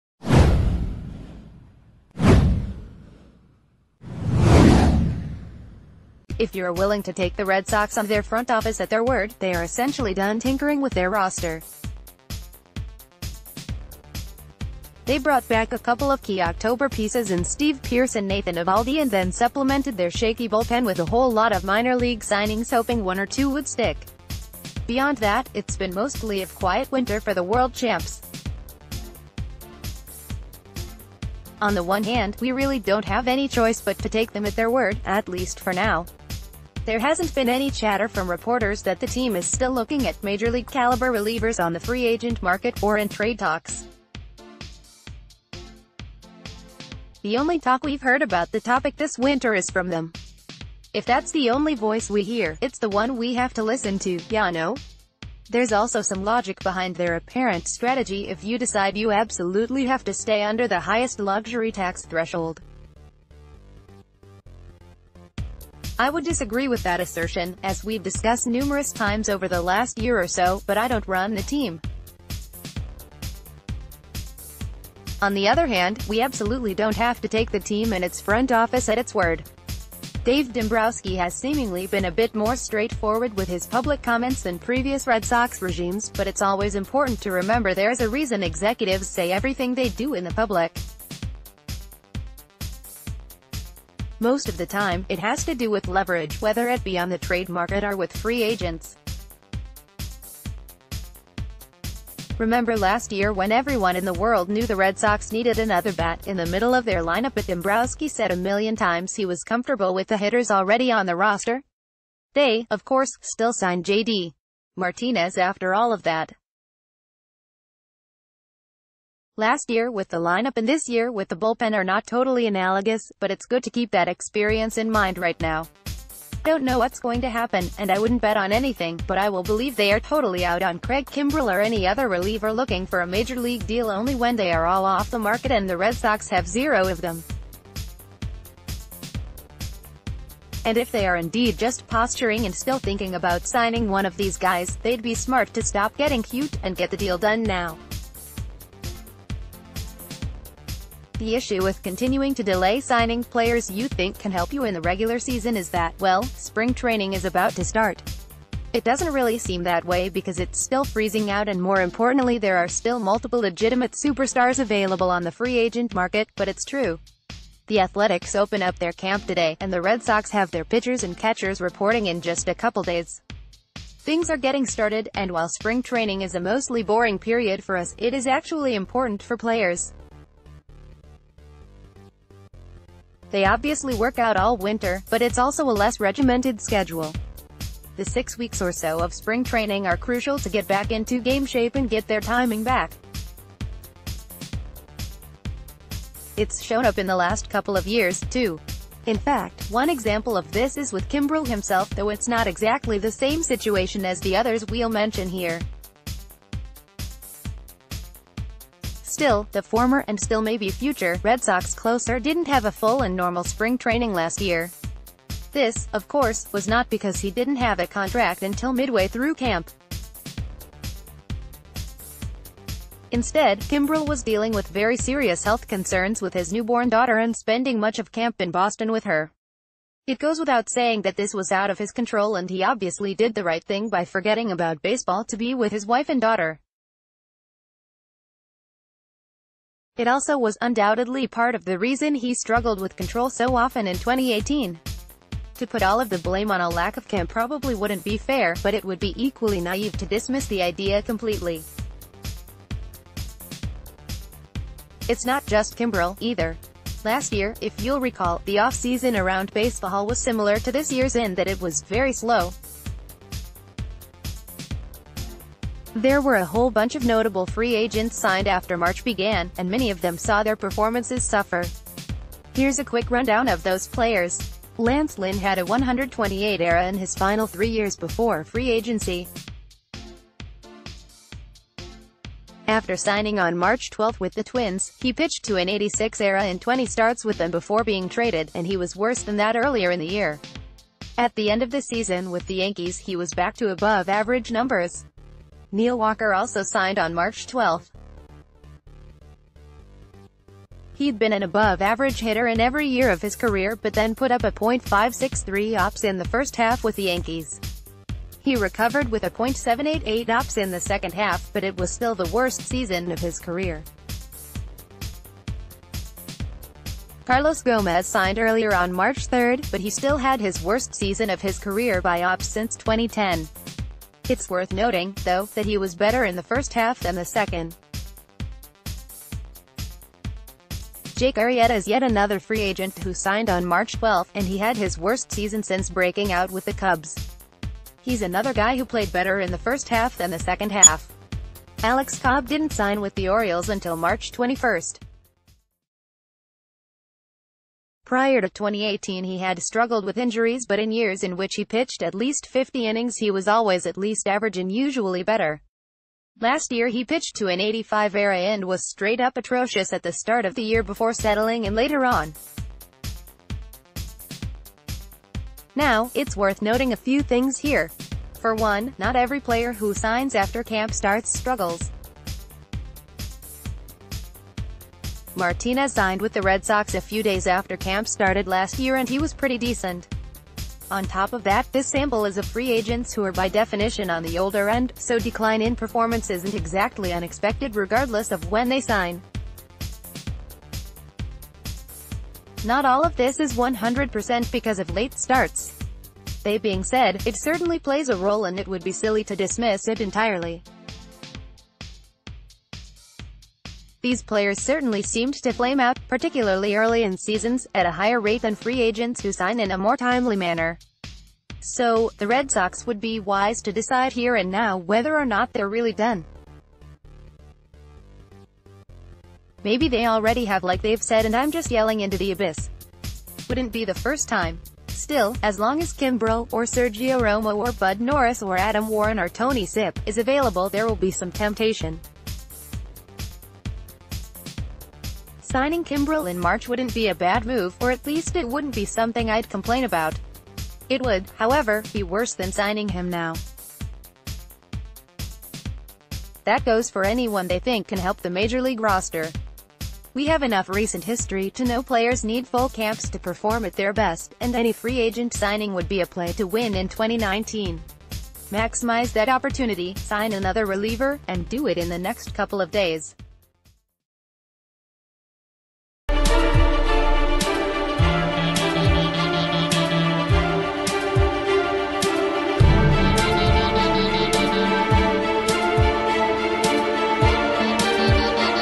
If the Red Sox have another move to make, they have to make it now
If you’re willing to take the Red Sox and their front office at their word, they are essentially done tinkering with their roster. They brought back a couple of key October pieces in Steve Pearce and Nathan Eovaldi and then supplemented their shaky bullpen with a whole lot of minor-league signings hoping one or two would stick. Beyond that, its been mostly a quiet winter for the World Champs. On the one hand, we really don’t have any choice but to take them at their word, at least for now. There...